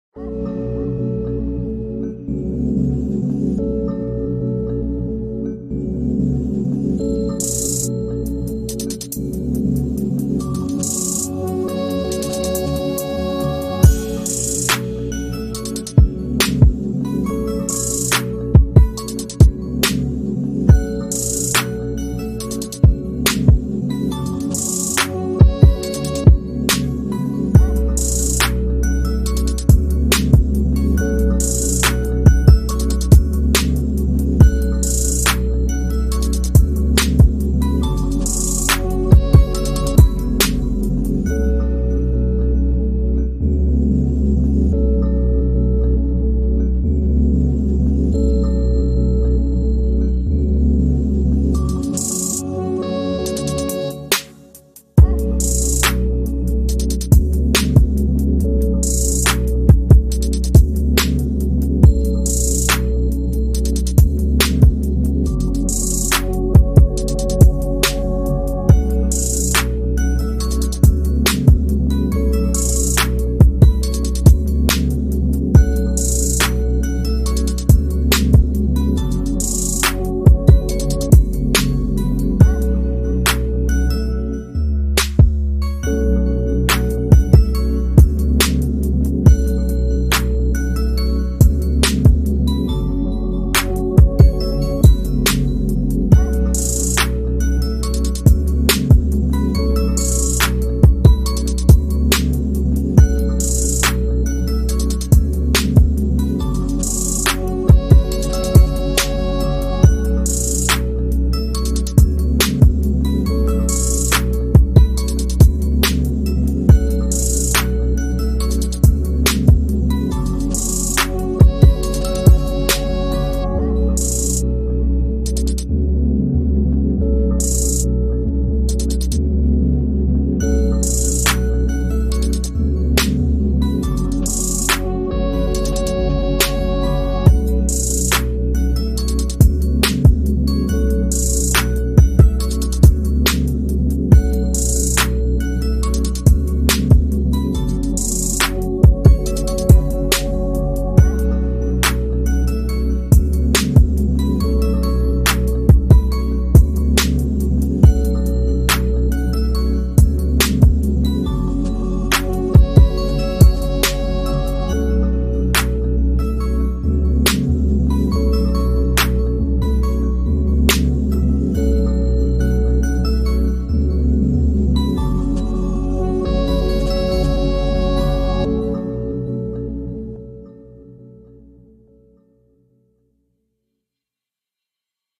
🤤This Jeep is SEXY. Tint Factor in Wetumpka Alabama hooked this up
Tinting a Jeep in Wetumpka Alabama